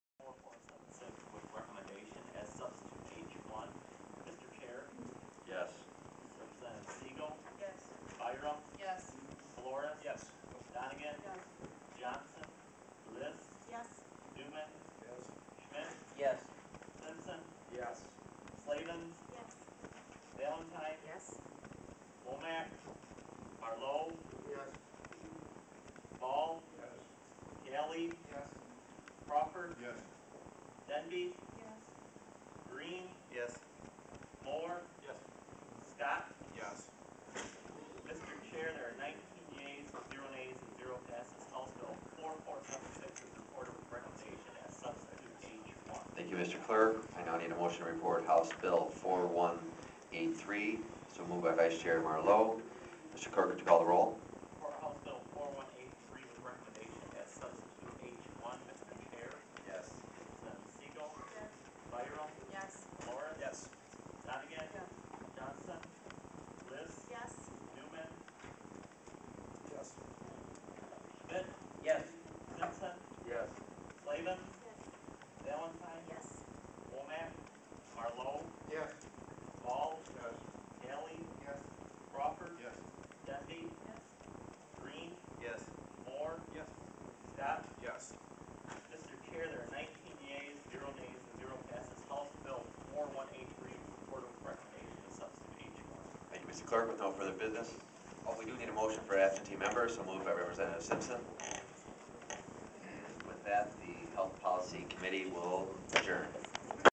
The Vote Is Unanimous! House Health Policy Committee 6/9/09 Autism Insurance Legislation Michigan
The Vote Is Unanimous! House Health Policy Committee 6/9/09 Autism Insurance Legislation Michigan. 19-0